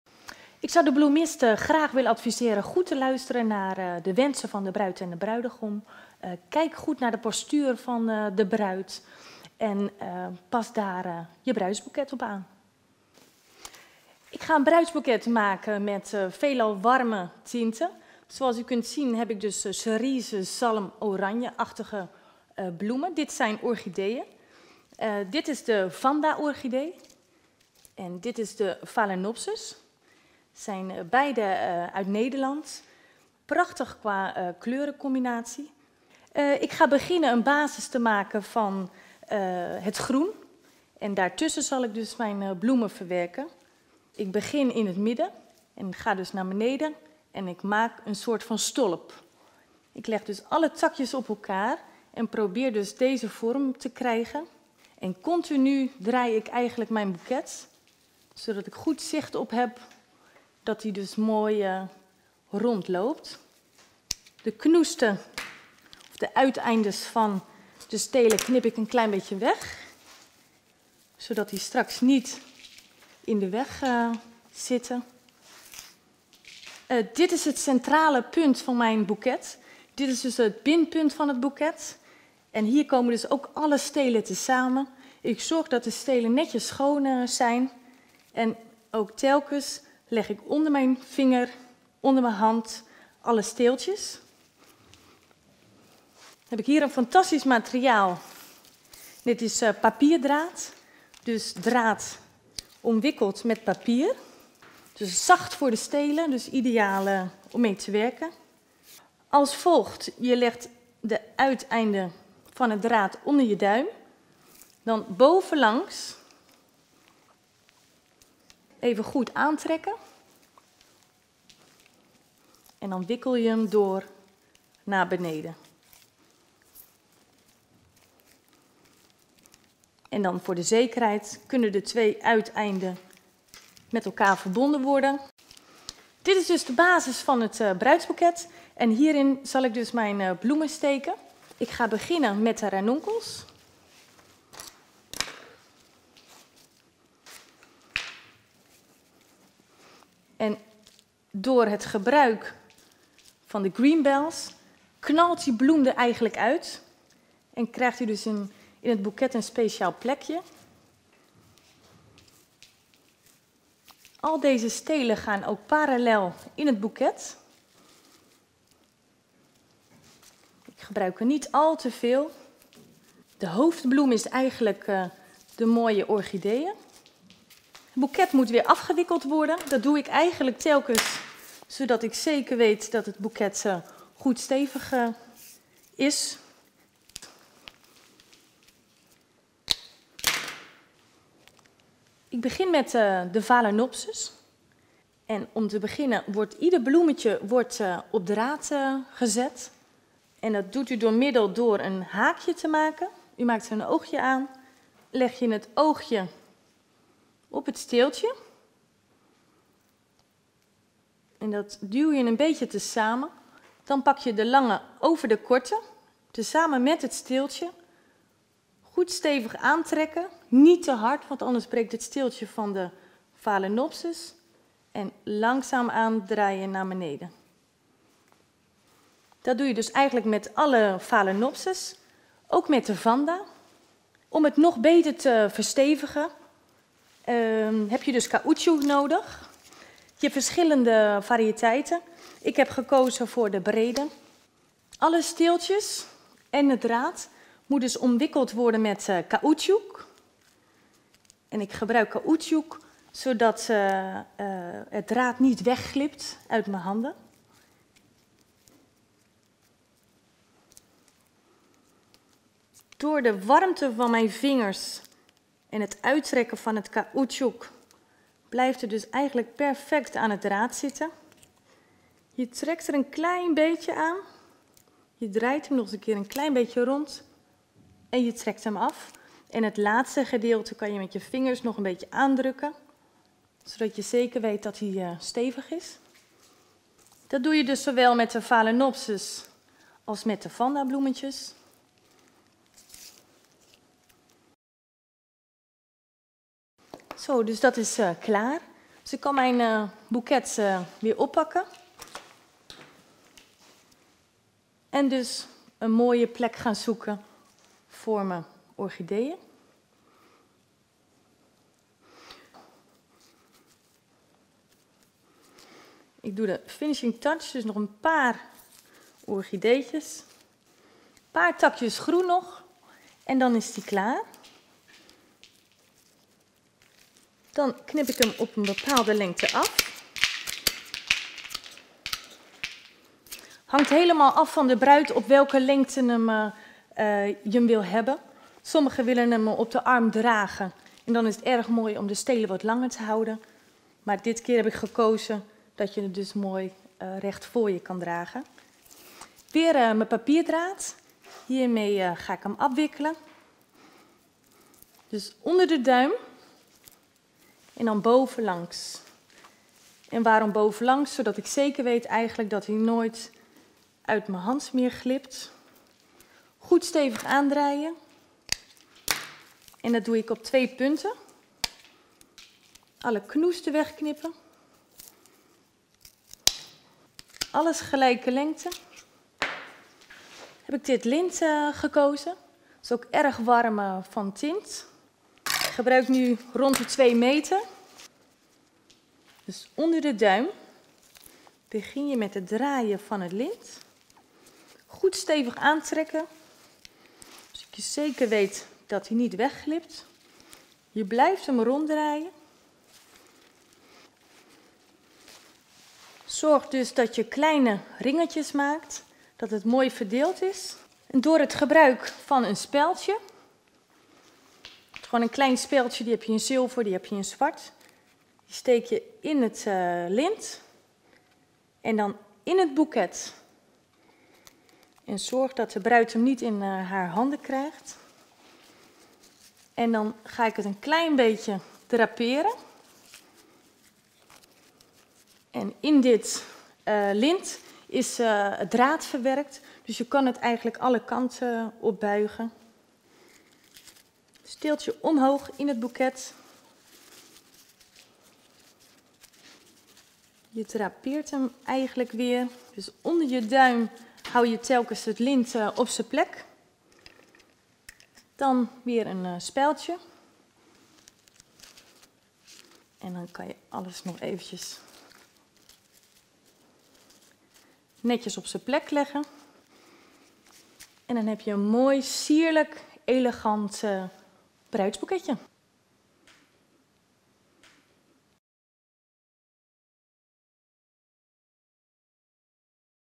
Atelier d'inspiration 'Mariage' de Desiree Glasbergen
Visionnez ici l'atelier d'inspiration 'Mariage' de l'artiste floral de haut niveau Desiree Glasbergen. Dans le bouquet de mariée, elle travaille la Vanda et le Phalaenopsis.

La Vanda et le Phalaenopsis sont disponibles tout au long de l'année.